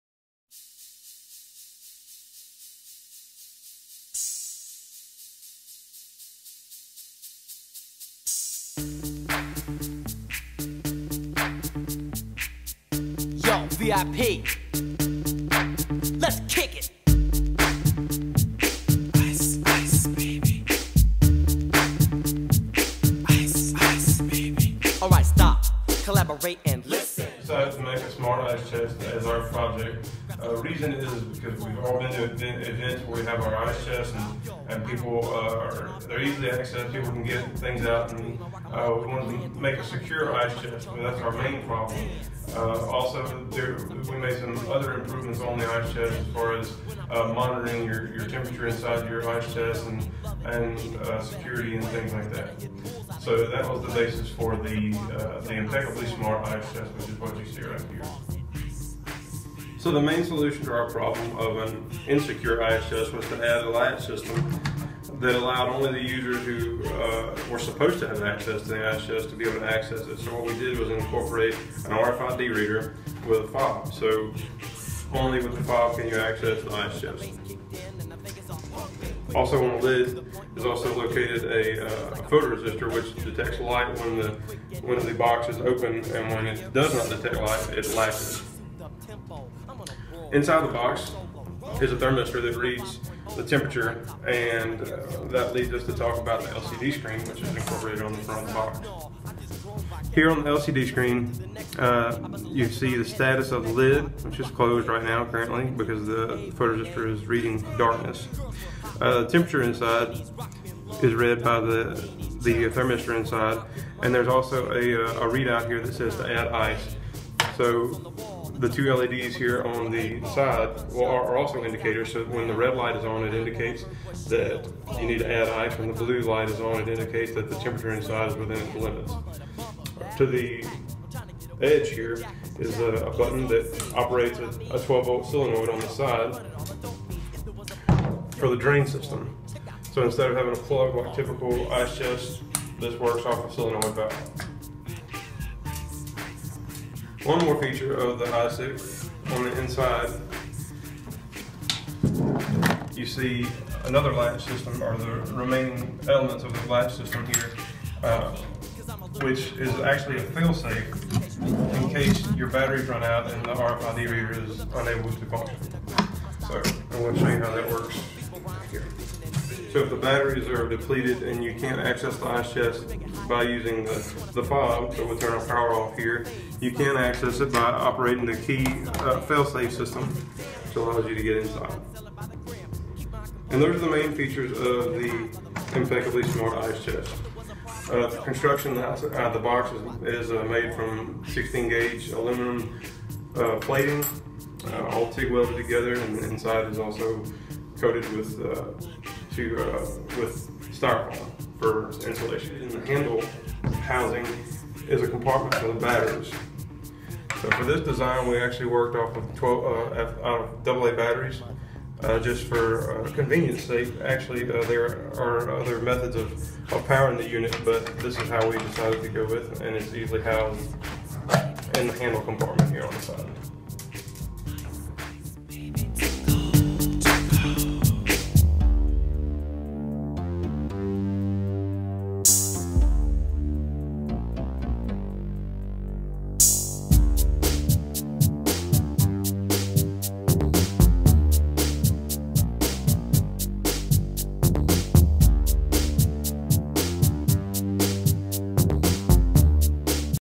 ISIC: The Impeccably Smart Ice Chest - Reeves - Spring 2014
This is our project for ENGR 122 at Louisiana Tech, The Impeccably Smart Ice Chest. Our group includes - Jarrett Bailey, Matthew Downhour, Corey Jett, and Custer Fontana. The ISIC is a smart ice chest that reads temperature inside and displays on an LCD screen on front. The LCD screen also displays the lid status (open or closed). There are two LEDs on front that indicate when to add or not to add ice. It also has an RFID secure locking system to keep the contents of the chest secure. Finally, a solenoid valve drain system was implemented to replace the typical plug found on many ice chests. There is a key operated failsafe system in case of power loss. The box is made of 16 gauge aluminum and lined with styrofoam insulation. Ladies and gentlemen, the Impeccably Smart Ice Chest!